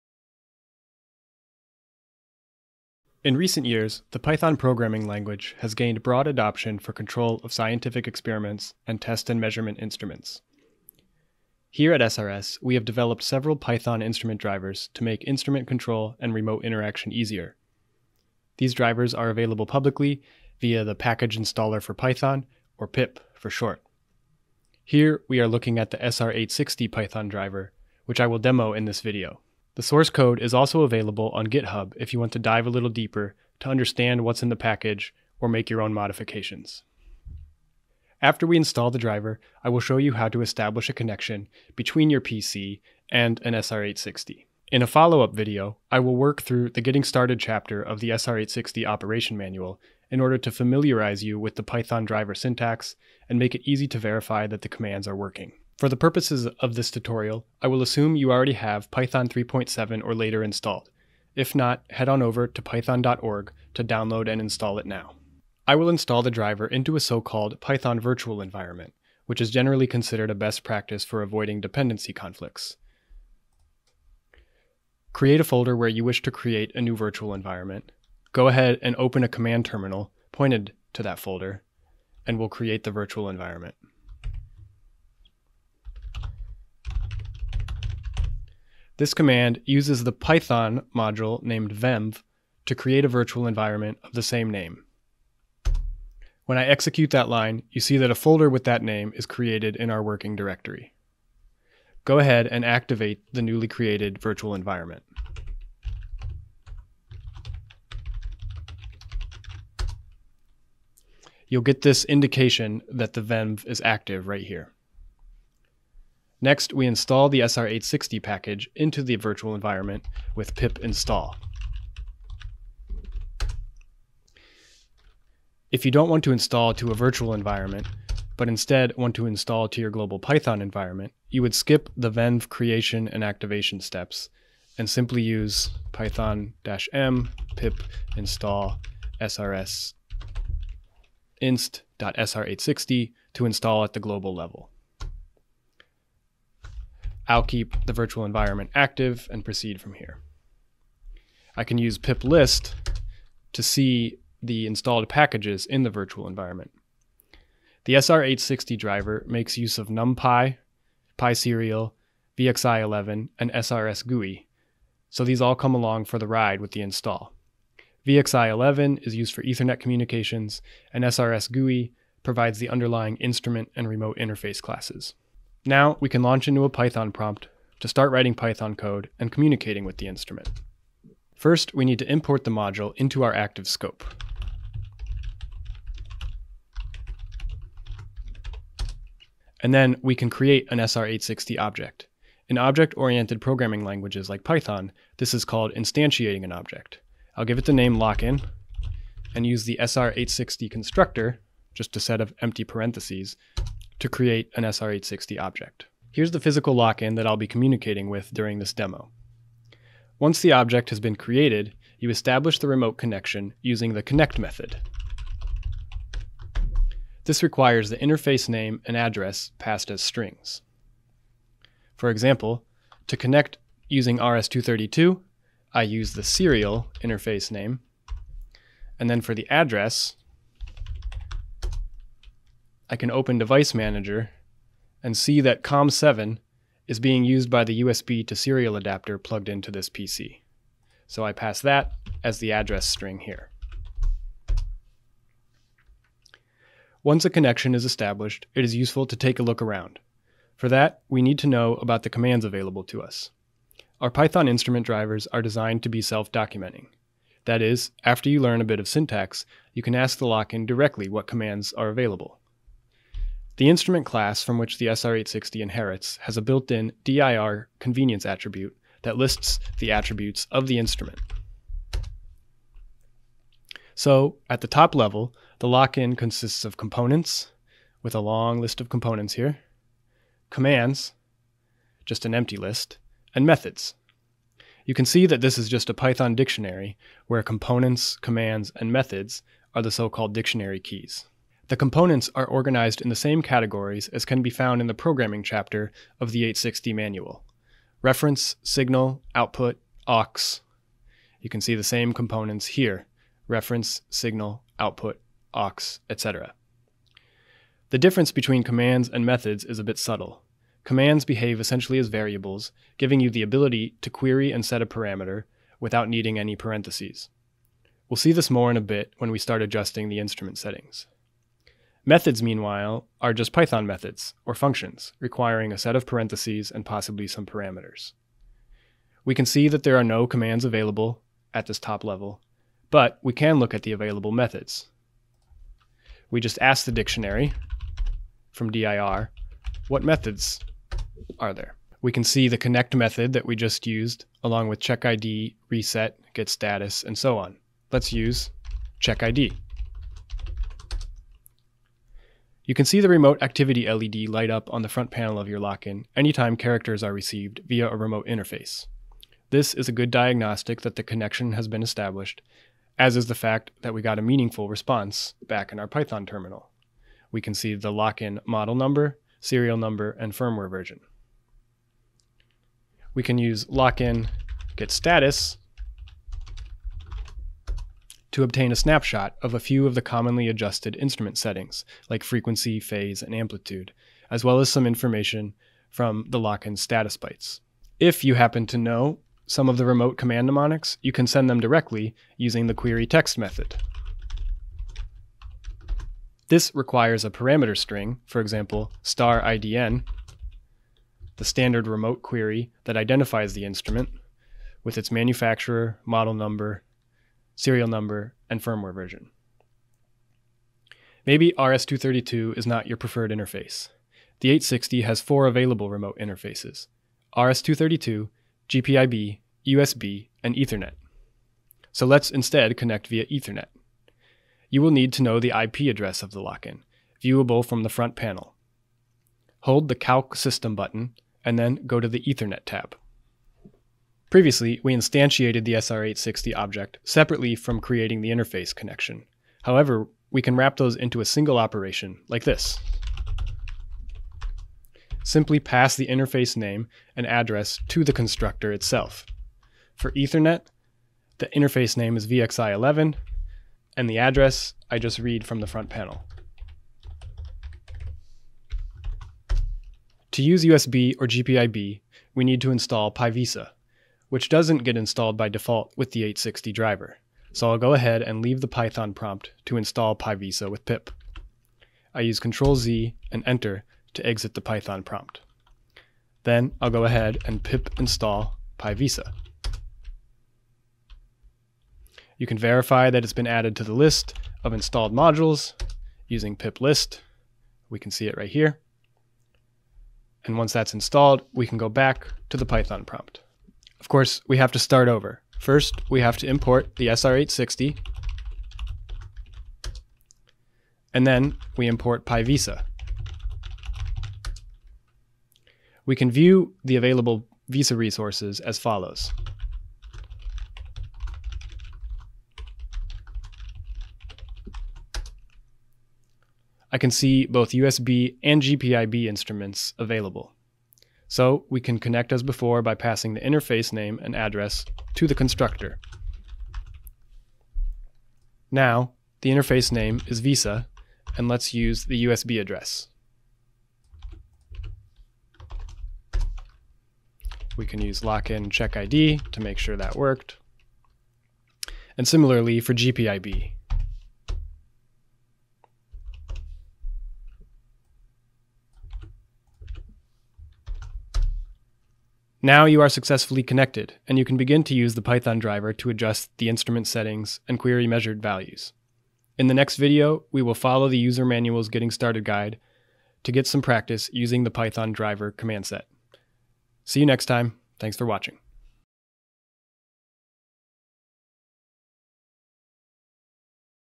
Installing the SR860 Python Driver
If you have an SRS SR860 or SR865A Lock-in amplifier that you would like to control with Python from a PC, you can use our srsinst.sr860 Python driver.
This video (Part 1 of a 3 part series) will teach you how to install the driver and start communicating with your instrument.

Chapters:
00:00 Introduction
01:05 Installation into a Python virtual environment
02:55 The Python prompt
03:26 RS-232 connection
04:06 Self-documenting code
07:02 Ethernet connection
07:41 USB and GPIB connection with PyVisa
09:13 Wrap-up